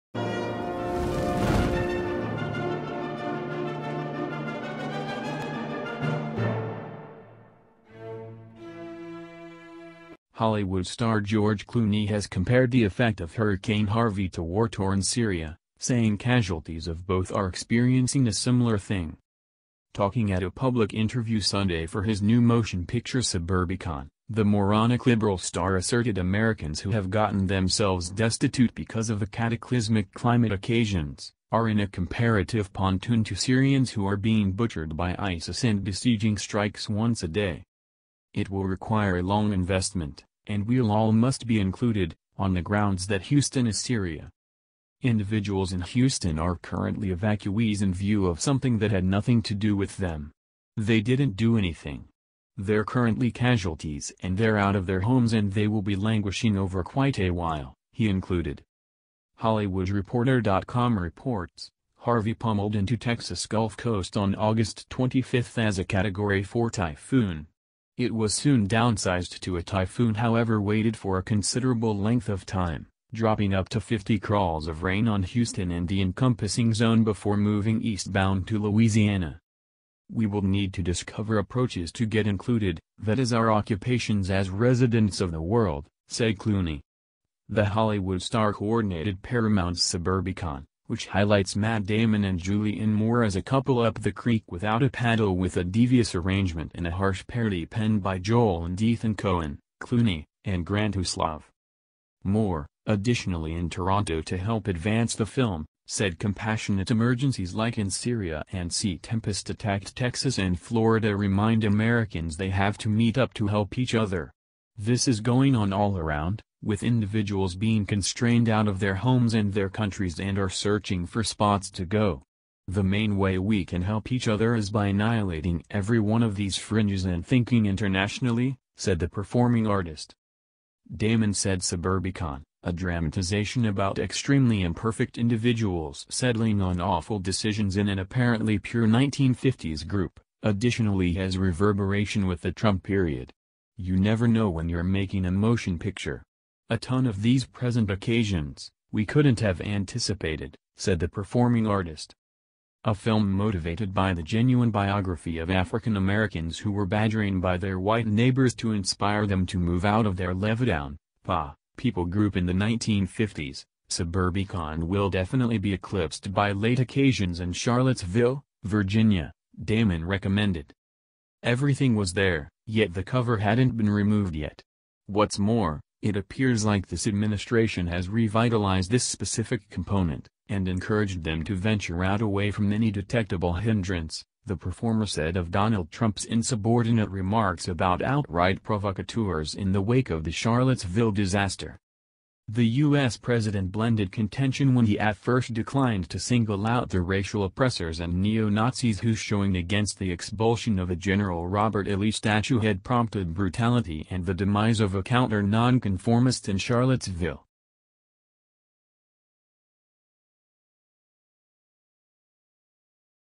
George Clooney Compares Hurricane Harvey To War torn Syria | Must-See
Hollywood star George Clooney has compared the effect of Hurricane Harvey to war-torn Syria, saying casualties of both are experiencing a similar thing.